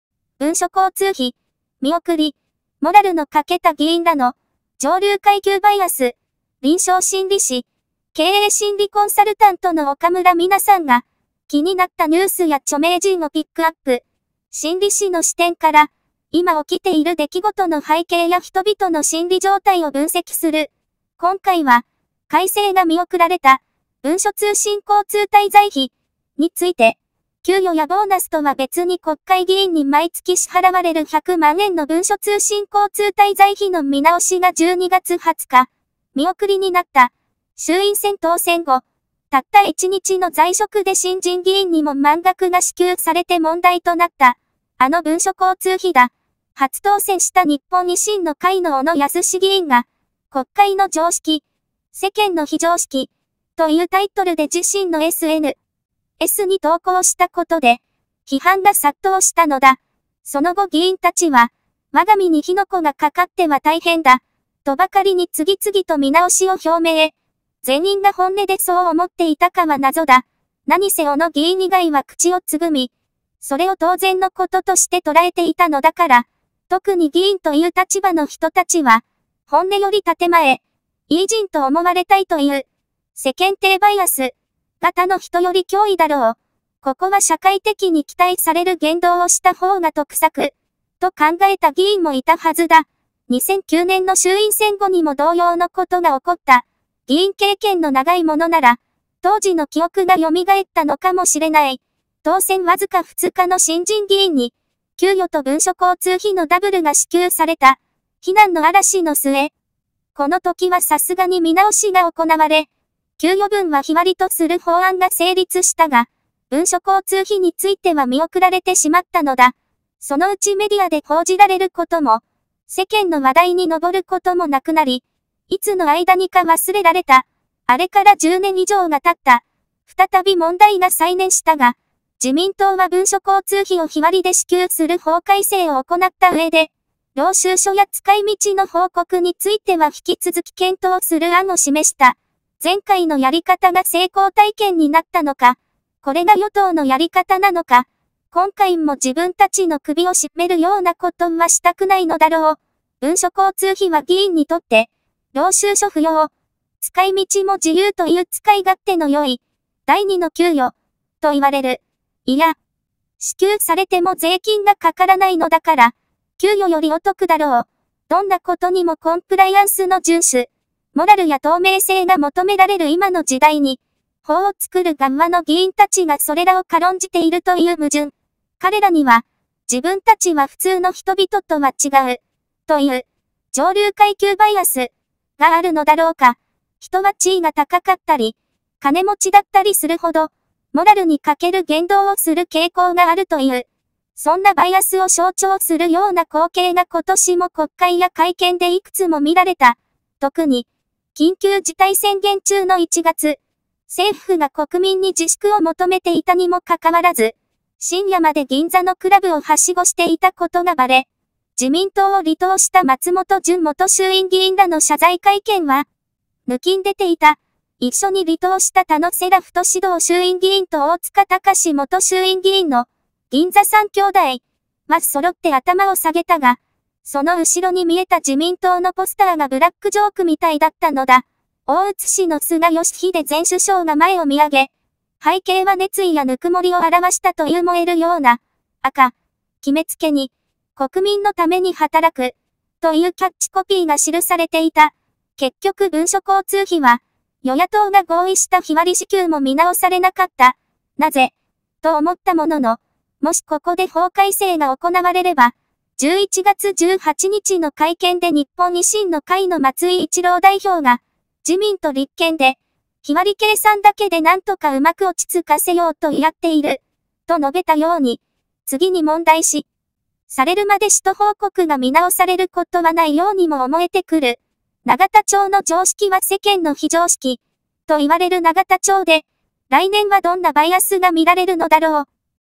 議員らは”上流階級！？
「文書交通費」見送り モラルの欠けた議員らの”上流階級バイアス”